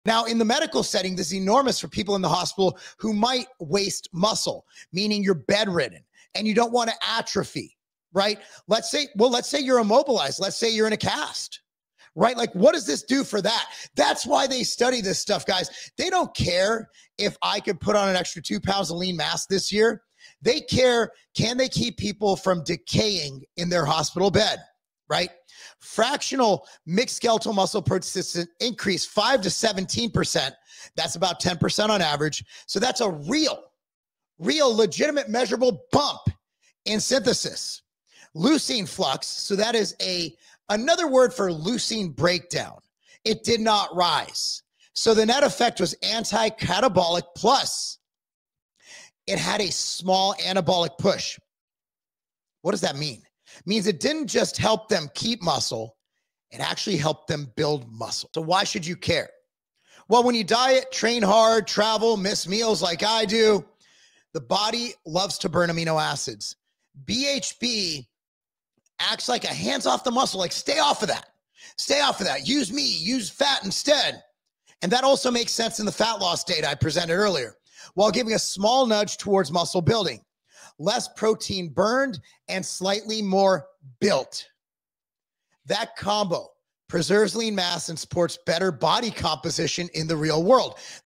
Hospital Muscle Decay Solved? BHB's Anabolic Power REVEALED!
Discover how BHB can help preserve muscle mass during dieting, hard training, and travel. BHB acts like a hands-off signal to muscles, encouraging the body to use fat instead. Less protein burned and slightly more built helps maintain better body composition.